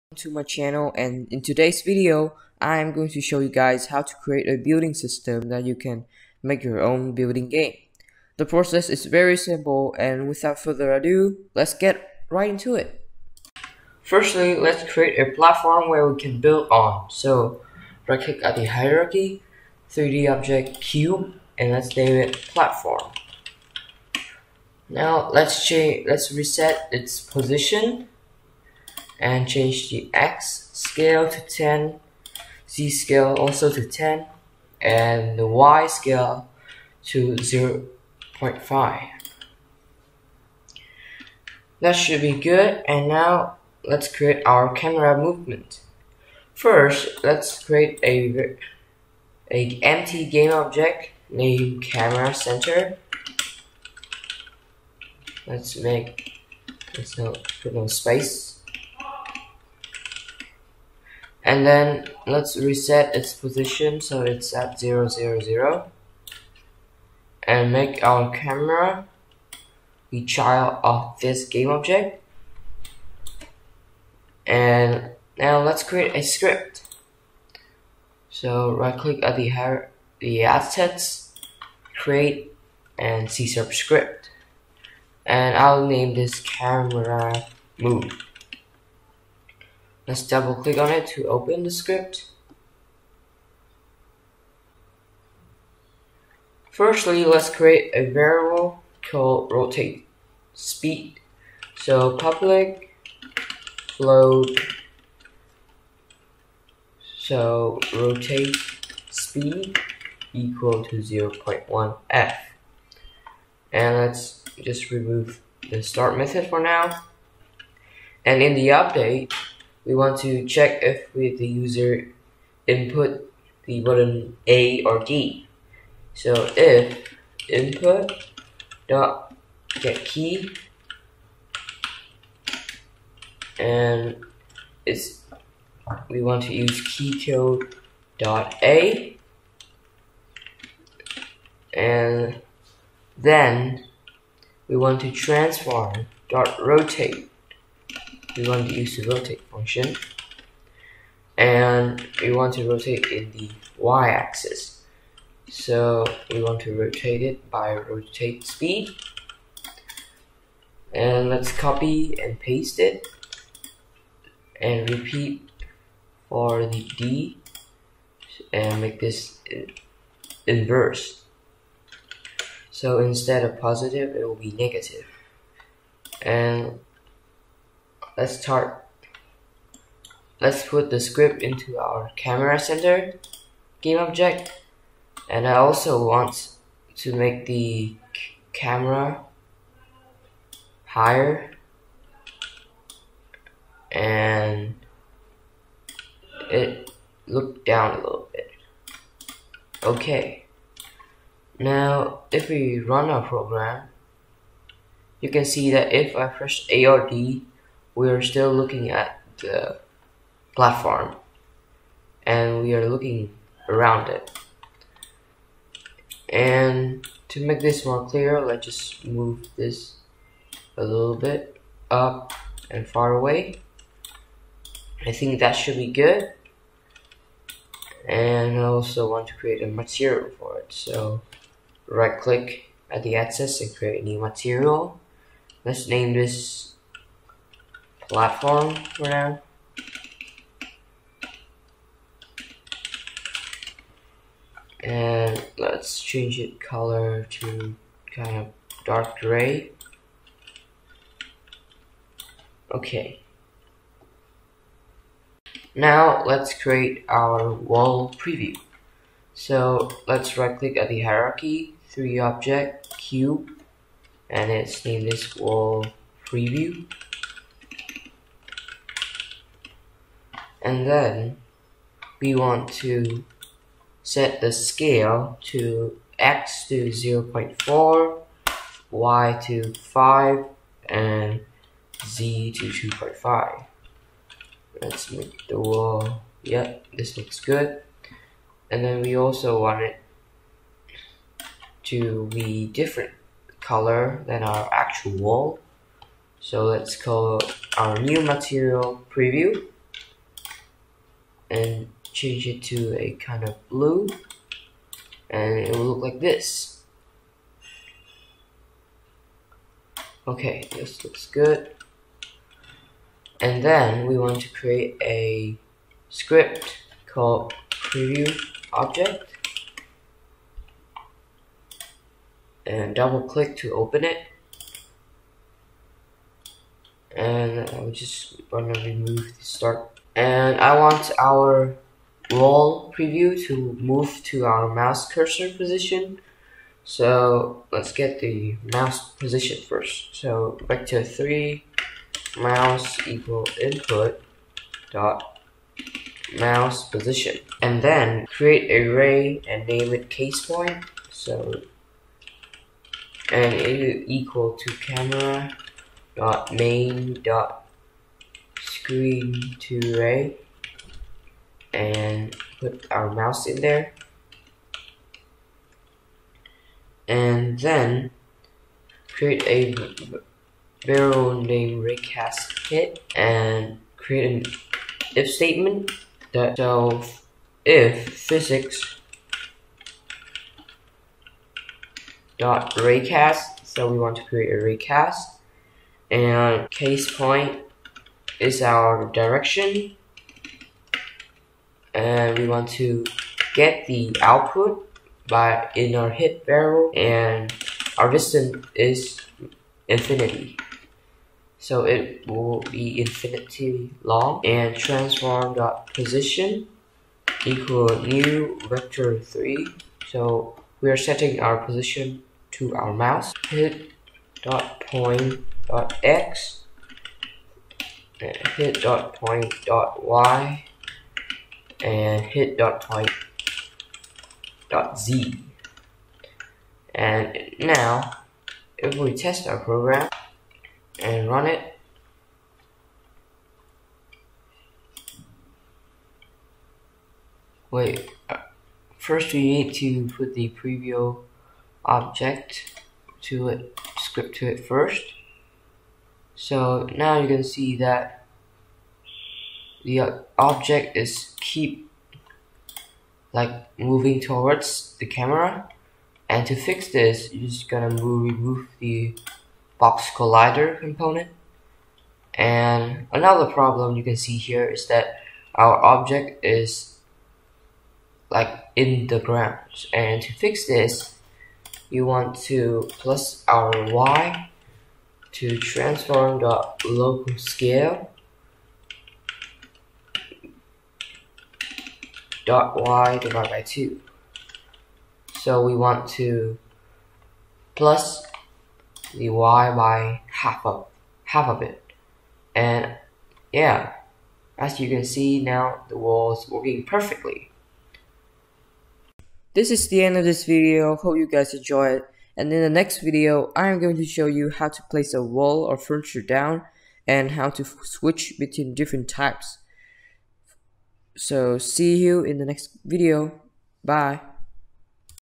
How to create a Building System with Unity! EP1
In this video, I will show you guys how to create a building system using Unity. This project is very fun and you can always add more furniture and more things to place to create your own building game!